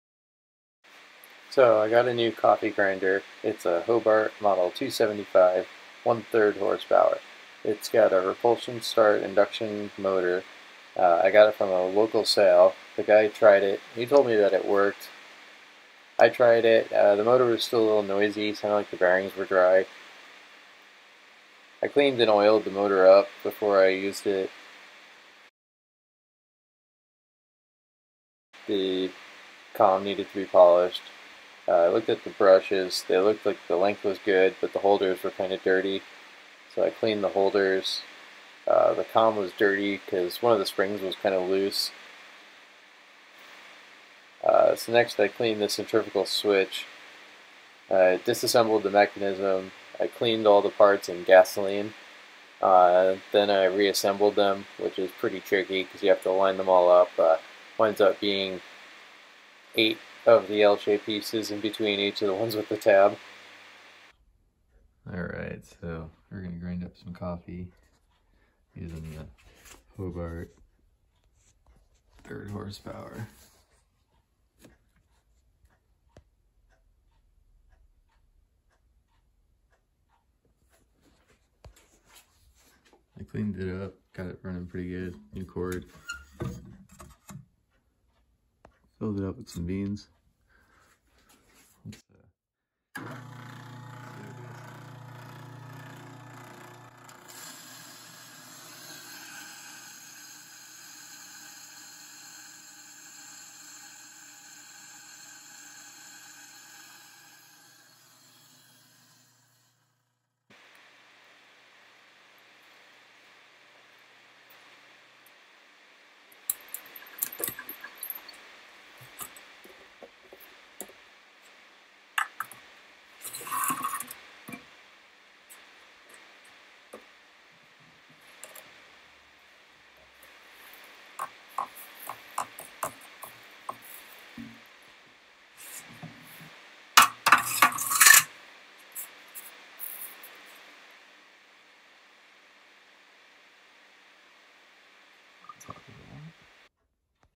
Hobart Coffee ( Mill) grinder from 1920s cleanup and running Model 276
old video of my coffee grinder, Hobart 1/3 hp, I use it every week even in 2021

Same model that Hand Tool Rescue restored!